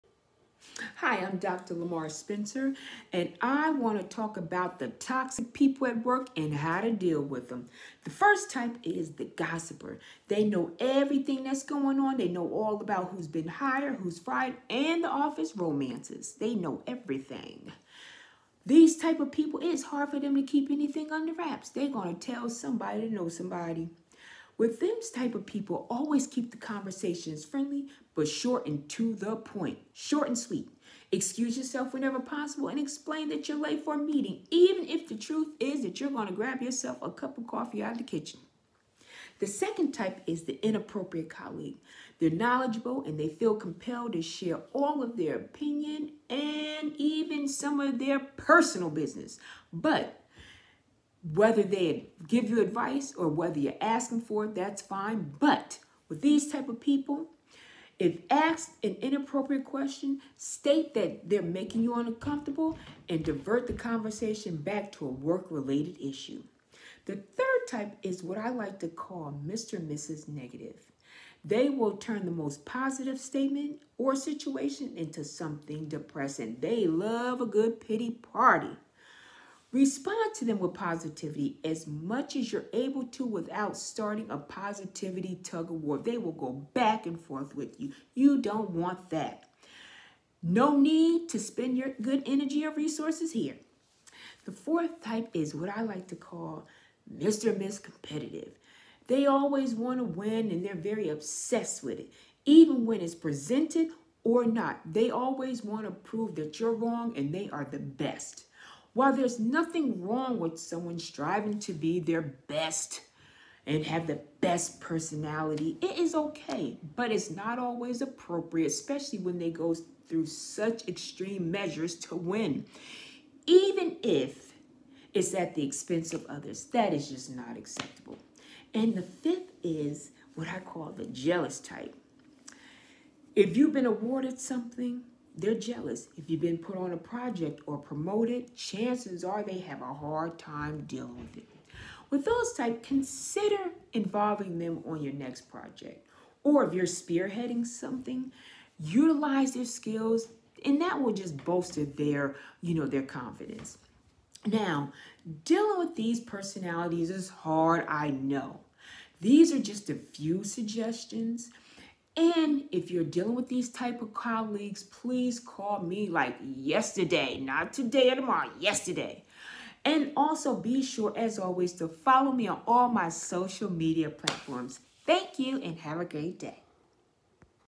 🔥The Toxic People at Work and How to Deal with them🔥 | Dr. Lamarr Renee Spencer
🔥The Toxic People at Work and How to Deal with them🔥

Toxic individuals are very real impediments to a productive and healthy work environment. They can contaminate and even change the mood of an entire team. But what should you do? Do you disregard them? Do you go up to them? How can you possibly change their toxic behavior if management is oblivious? In this video, the suggestions are only a start.